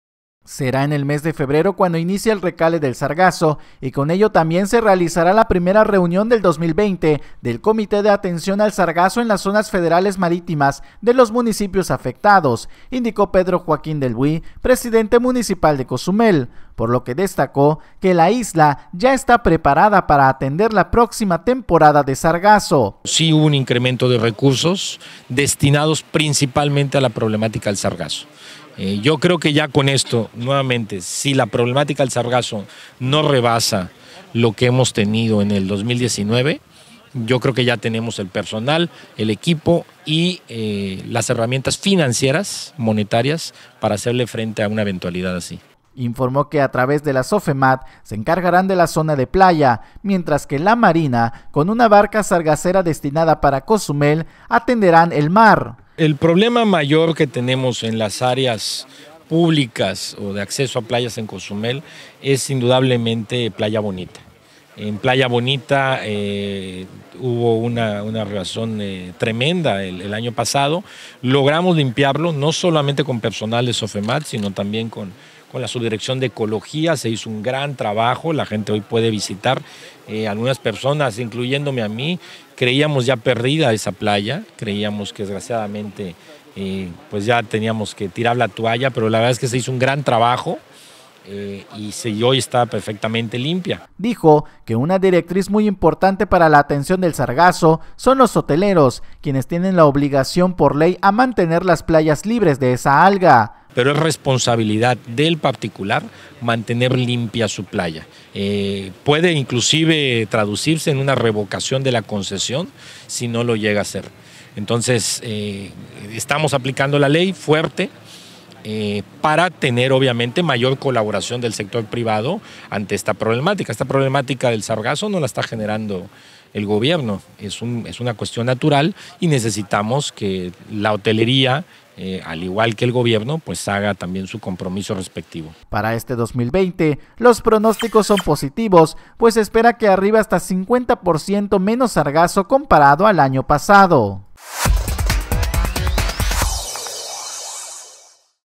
SE ESTIMA MENOS SARGAZO PARA ESTA TEMPORADA DE RECALE
Será en el mes de febrero cuando inicie el recale del sargazo y con ello también se realizará la primera reunión del 2020 del comité de atención al sargazo en las Zonas federales marítimas de los municipios afectados, indicó Pedro Joaquín Delbouis, presidente municipal de Cozumel, por lo que destacó que la isla ya está preparada para atener la próxima temporada de sargazo.